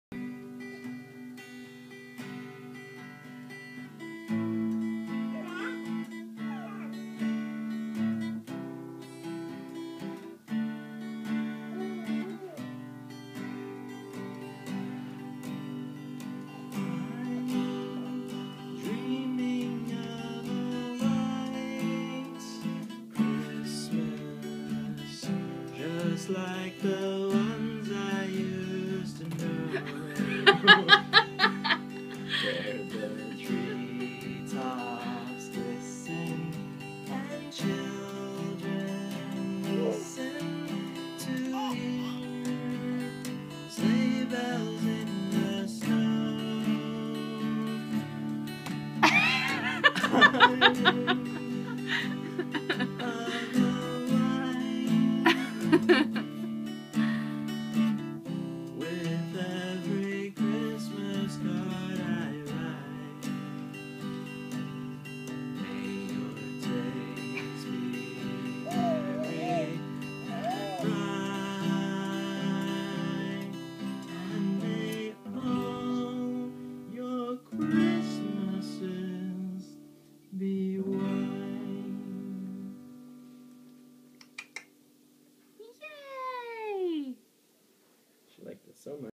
Ashtyn and daddy playing Christmas Carols
Ashtyn head banging to daddy playing the guitar!